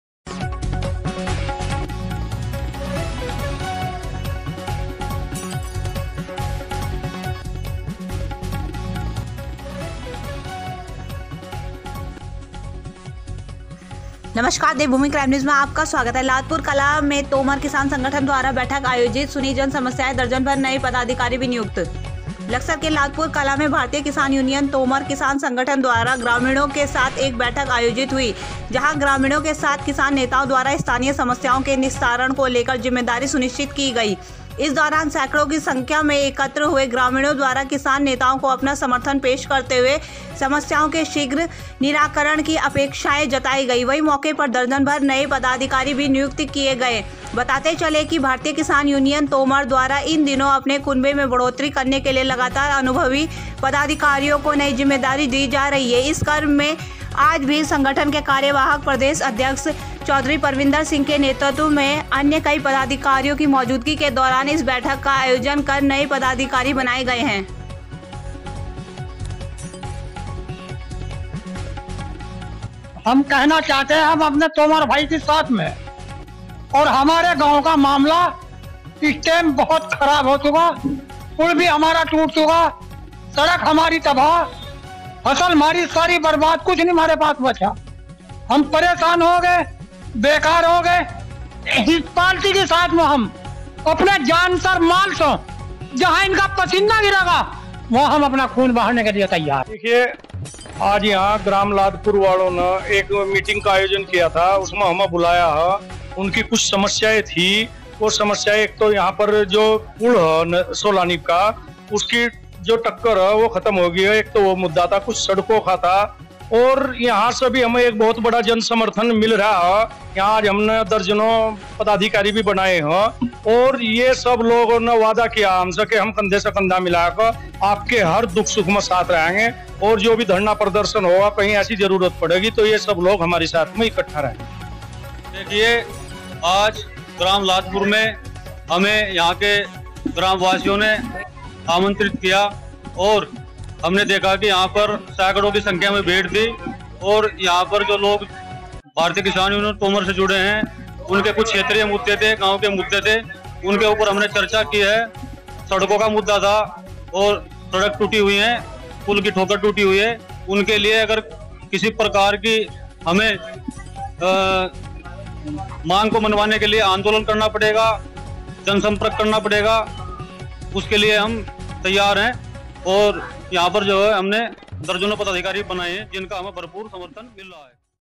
लादपुर कलाँ में तोमर किसान संगठन द्वारा बैठक आयोजित, सुनी जन समस्याएं। devbhoomi crime news,
लादपुर कलाँ में तोमर किसान संगठन द्वारा बैठक आयोजित, सुनी जन समस्याएं, दर्जनभर नए पदाधिकारी भी नियुक्त,